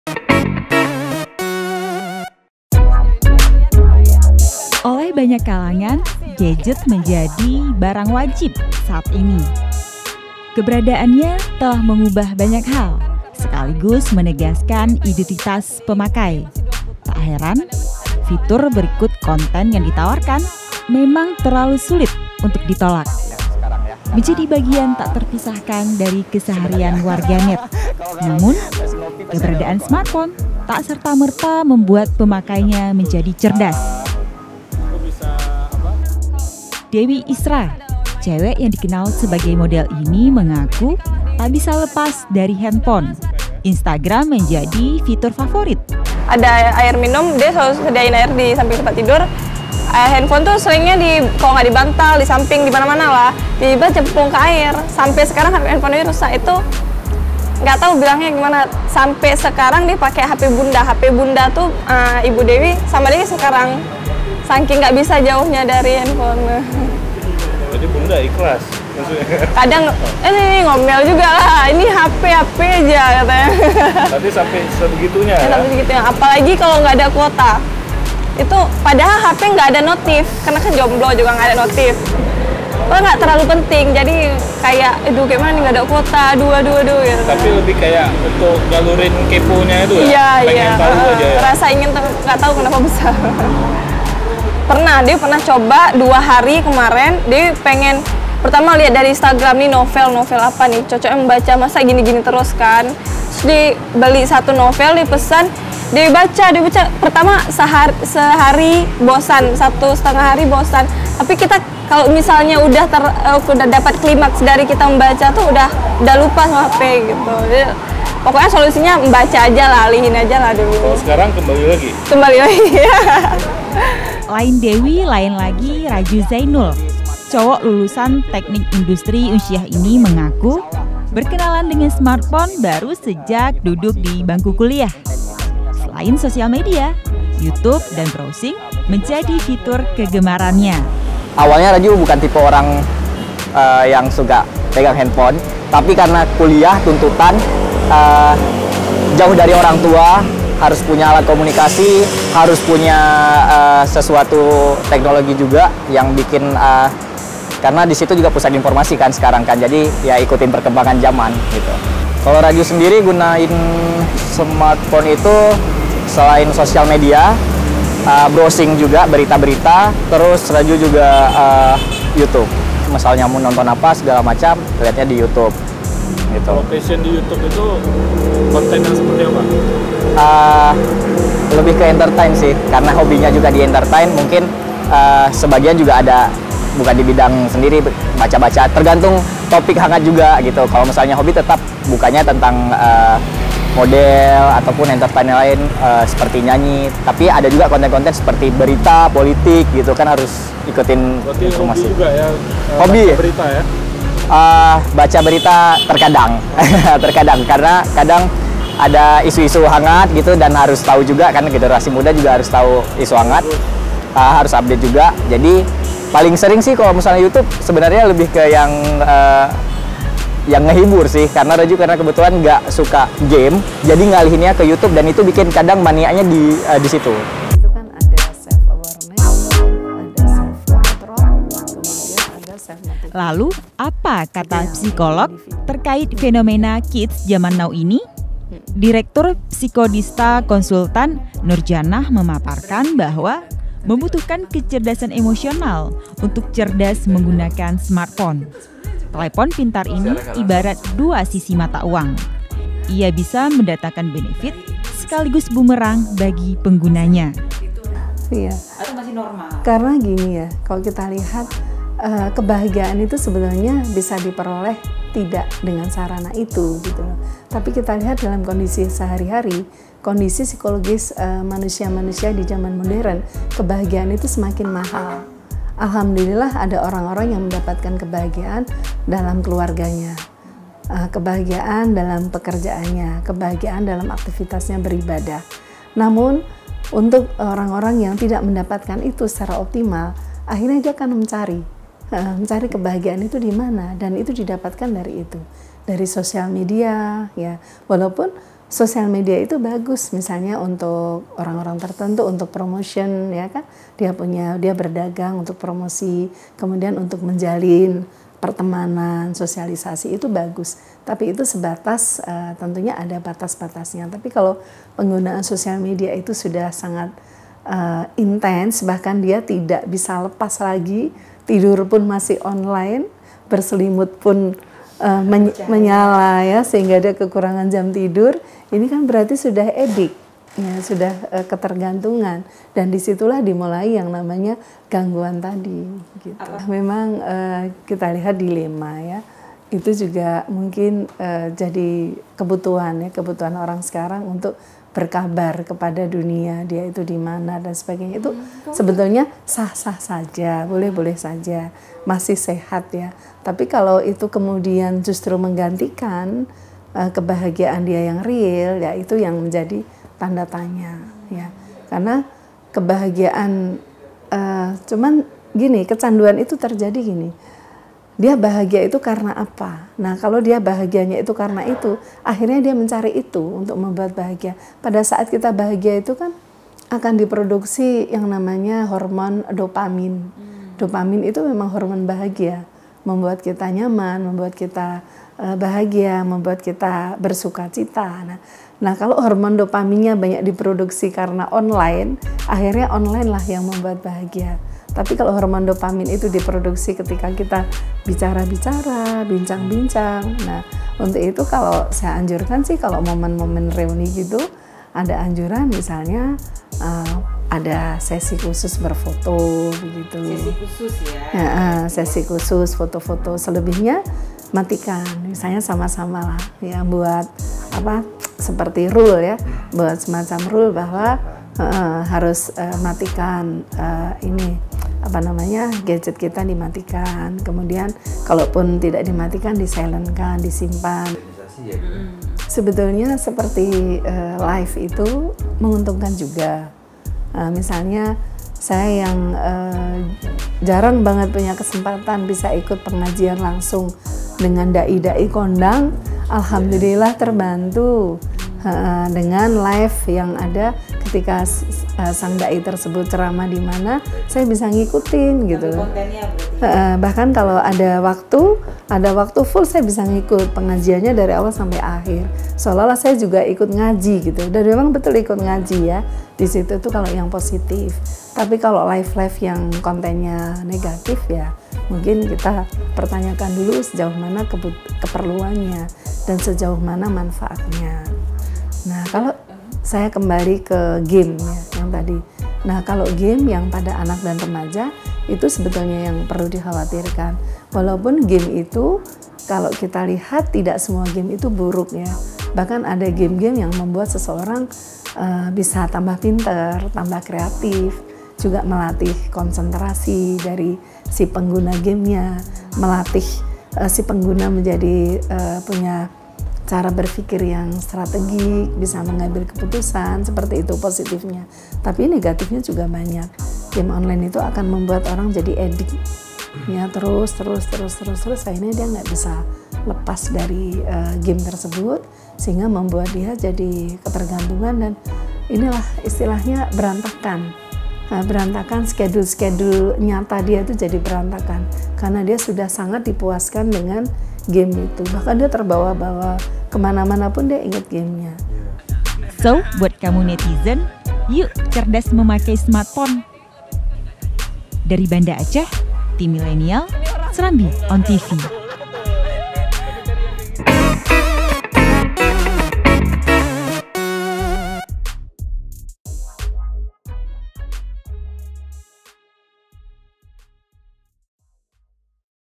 Cara Smart Menggunakan Smartphone
SERAMBITV.COM, BANDA ACEH - Oleh banyak kalangan, gadget menjadi barang 'wajib' saat ini. Keberadaannya telah mengubah banyak hal, sekaligus menegaskan identitas pemakai.
Tak heran fitur berikut konten yang ditawarkan, memang terlalu sulit untuk ditolak. Menjadi bagian tak terpisahkan dari keseharian warganet.

Namun keberadaan smartphone tak serta merta membuat pemakainya menjadi cerdas. Edisi millenials pekan ini, mengupas sisi keberadaan gawai.

Dewi Isra, cewek yang dikenal sebagai model ini mengaku tak bisa lepas dari handphone. Instagram menjadi fitur favorit gadis yang berkenalan dnegan smartphone sejak duduk di bangkku SMA ini.

Bakat keponya membuat ia sering belingsatan sendiri kalau kehabisan kuota.

Namun di sisi lain, ia juga rajin posting seputar dunia modeling yang digelutinya.

Pun begitu, menyadari kebiasaannya yang tak ubahnya kecanduan tersebut, ia mulai membatasi diri dengan mengalihkan minat ke hobi lainnya yaitu membaca buku.

Lain Dewi lain lagi Raju Zainul. Cowok lulusan Teknik Industri Unsyiah ini mengaku, berkenalan dengan smartpone baru sejak duduk di bangku kuliah.

Model yang berkeinginan menjadi conten creator ini tak menampik kalau smartphone sudah menjadi kebutuhan.

Untuk mengakali agar tidak kebablasan, ia pun membuat notifikasi kegiatan yang harus dilakukan. Wah! bisa dicontek nih guys.

Lalu apa kata psikolog terkait fenomena kids jaman now ini? Direktur Konseling Psikodista, Nurjannah memaparkan bahwa membutuhkan kecerdasan emosional untuk cerdas menggunakan smartphone.

Telepon pintar ini ibarat 'dua sisi mata uang'. Ia bisa mendatangkan benefit sekaligus bumerang bagi penggunanya. So buat kamu netizen, yuk! pintar memakai smartphone.